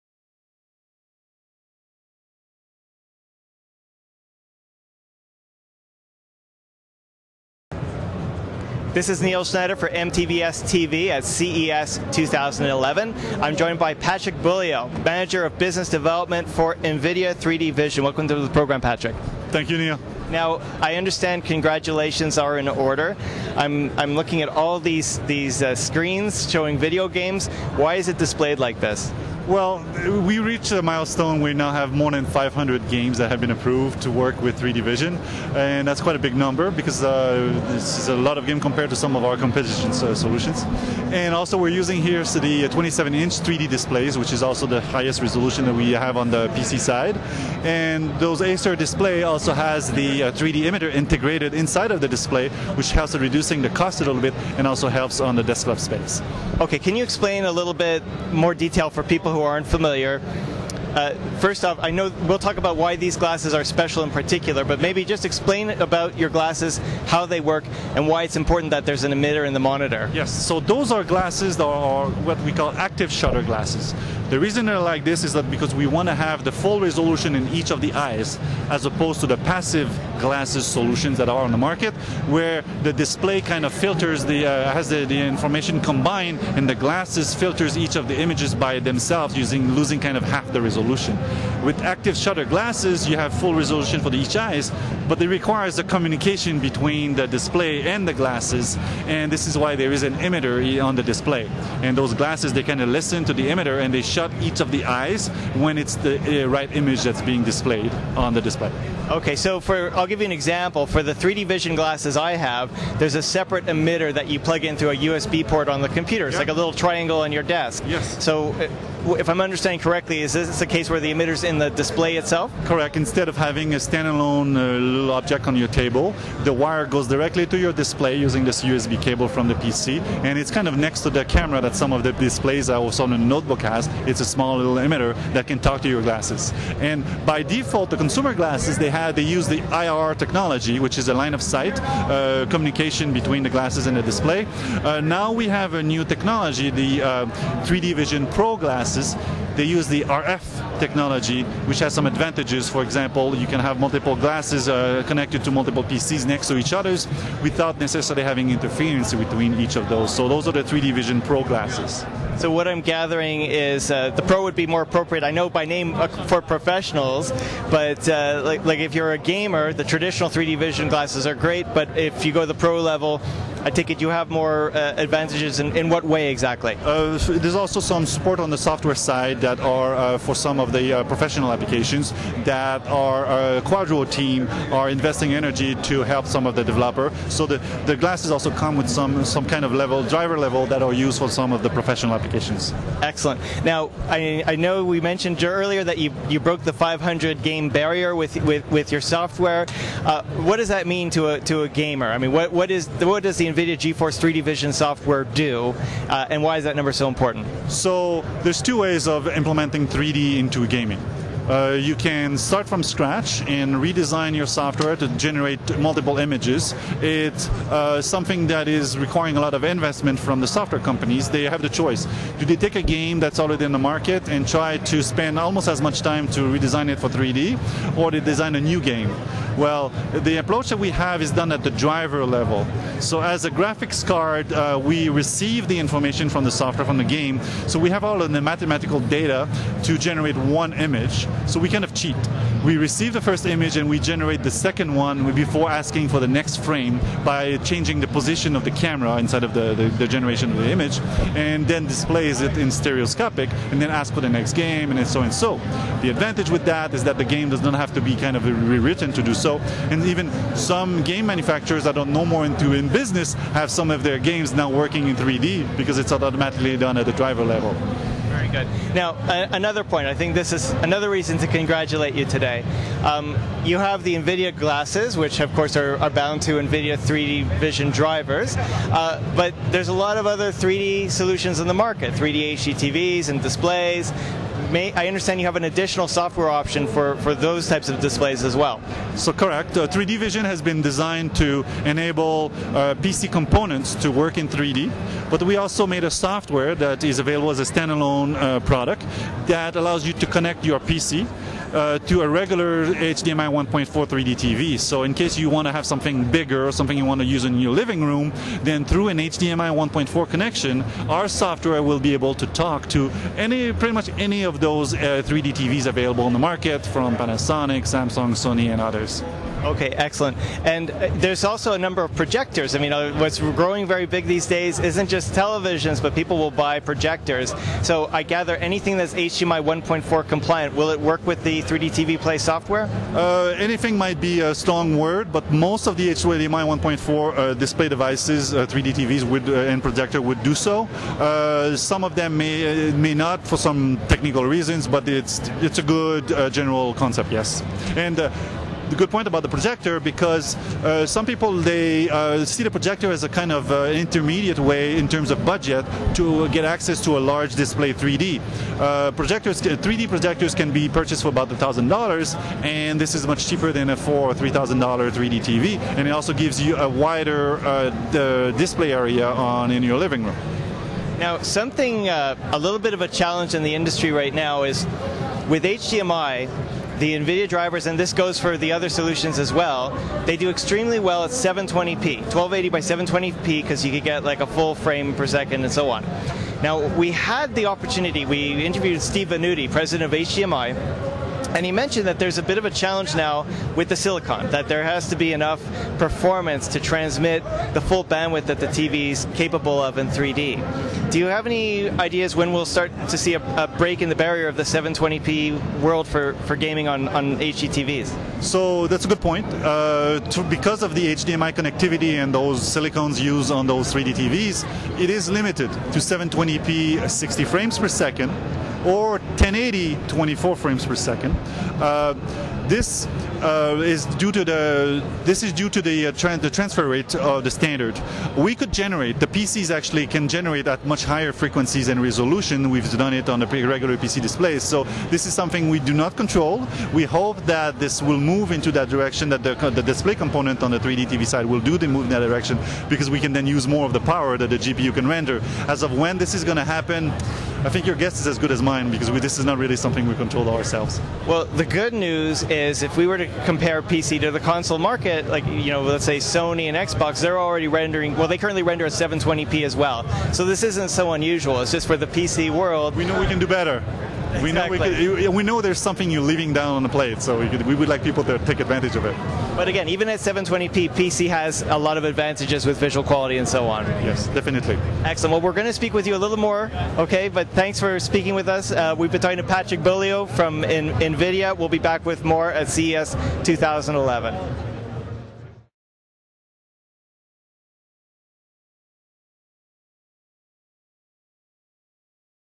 MTBS-TV: Nvidia at CES 2011, Part I
Neil Schneider interviews Patrick Beaulieu, Business Development Manager for 3D Vision Technologies at Nvidia.  Patrick talks about their RF based shutter glasses, 3DTV Play, and HDMI 1.4.